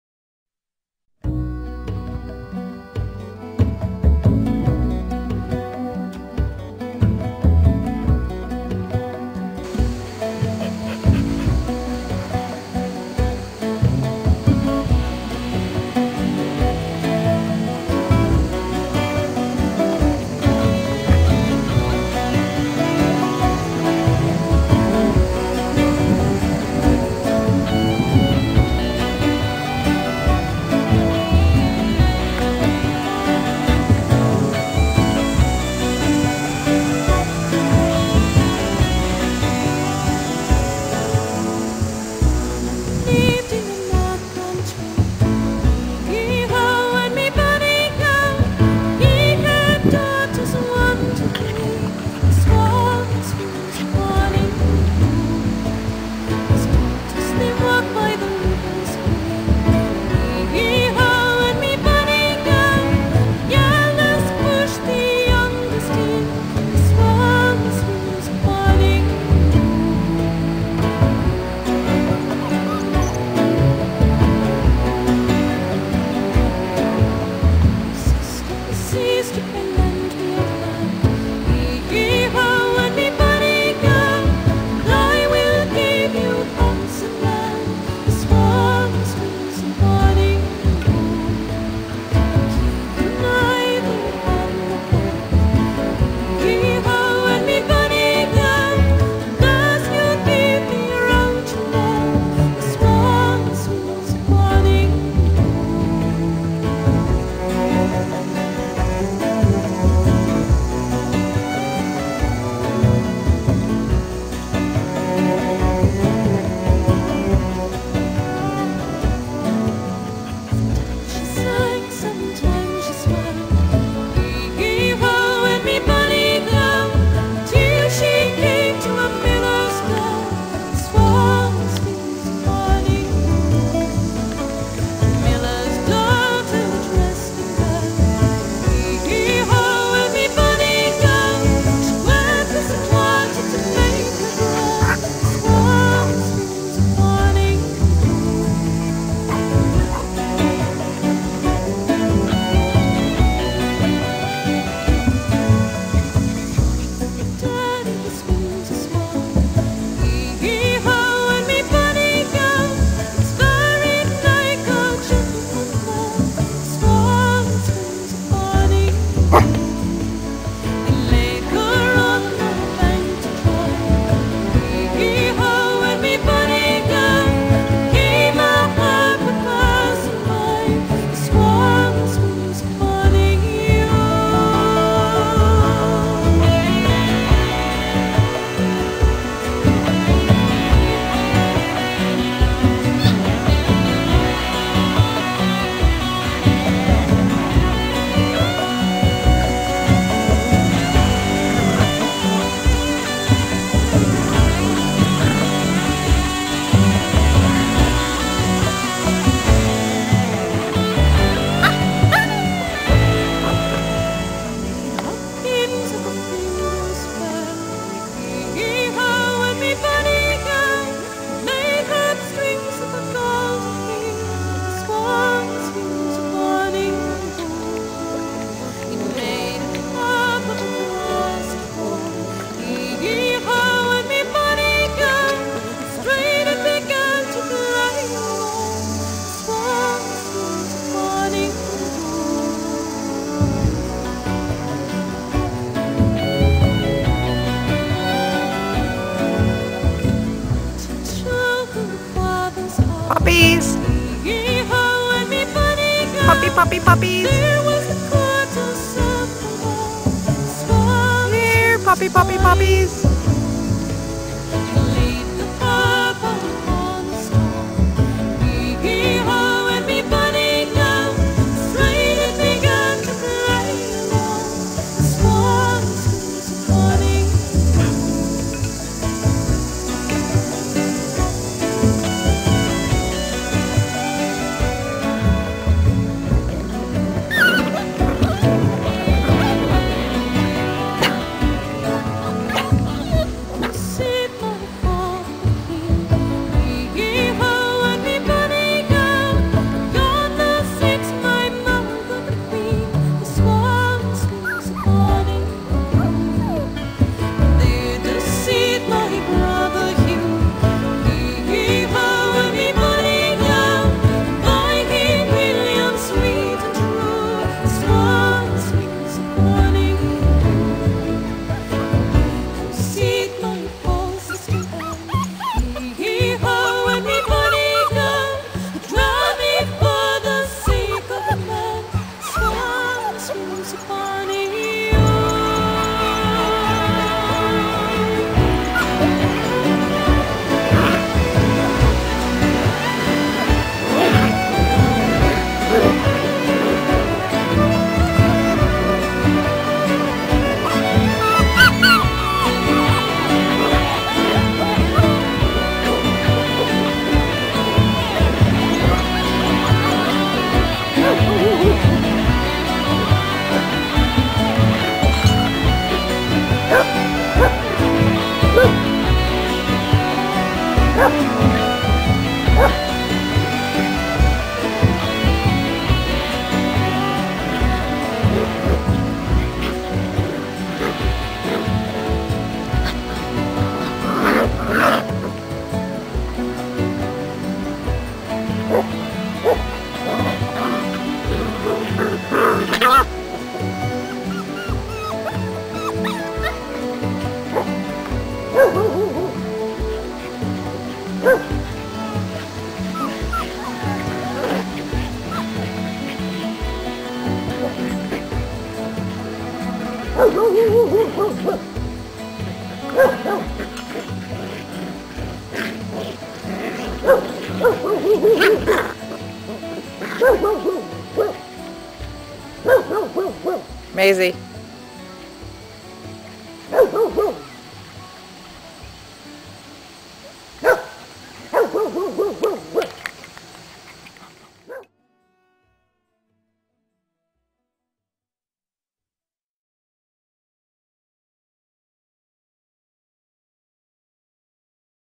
Litter of ENGLISH SPRINGER SPANIEL Puppies (6 Weeks Old) PLAYING w/ MOM
MAZY my love and her six black & white Springer beauties, enjoying the outdoors on a hot Summer day!! Her patience and sweet disposition are a joy to watch while she teaches, supervises and runs w/ her puppies. All play stops for a nursing break, then they romp, wrestle and explore some more!!! Everyday is WONDERFUL w/ PUPPIES!!! :-)